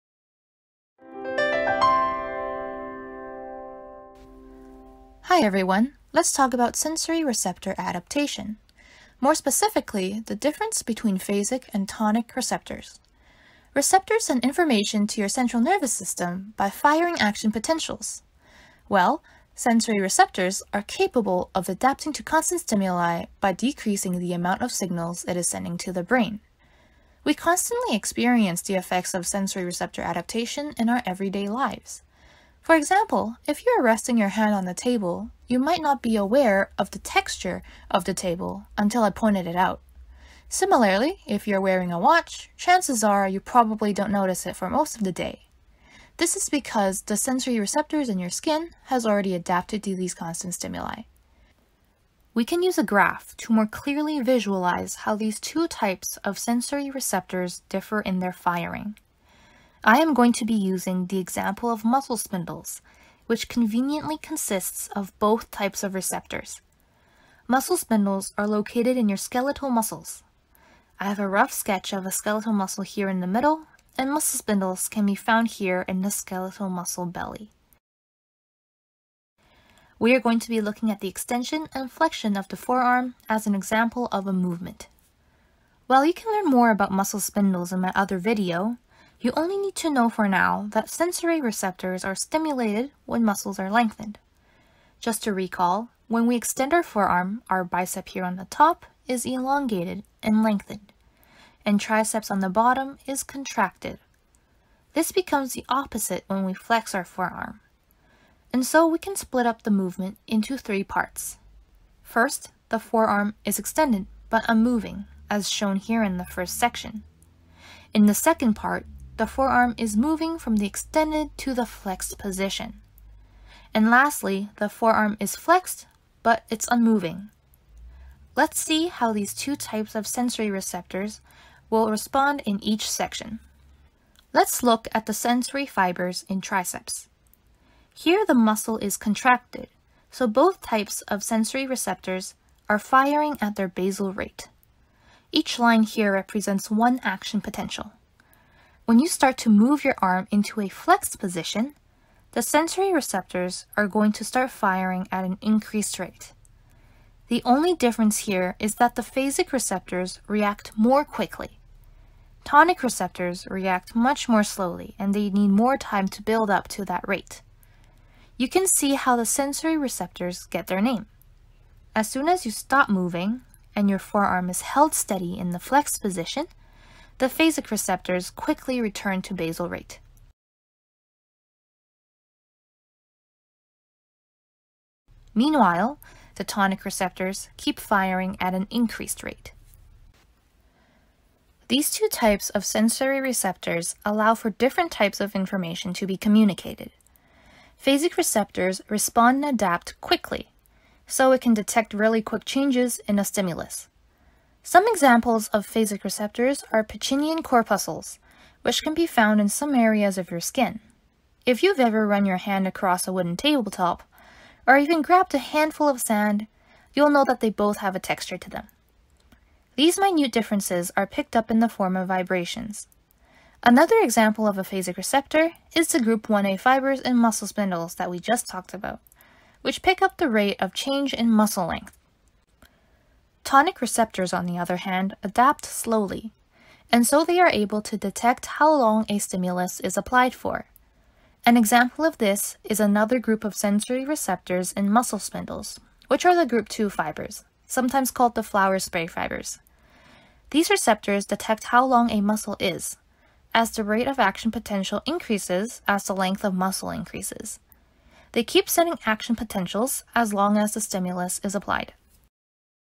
Sensory Receptor Adaptation: Tonic vs. Phasic Receptors
What's the difference between tonic and phasic sensory receptors? In this video, I use diagrams to explain the difference between the two in how they adapt to stimuli. Muscle spindles are used as the primary example, and the difference in action potential firing is explained in a graph.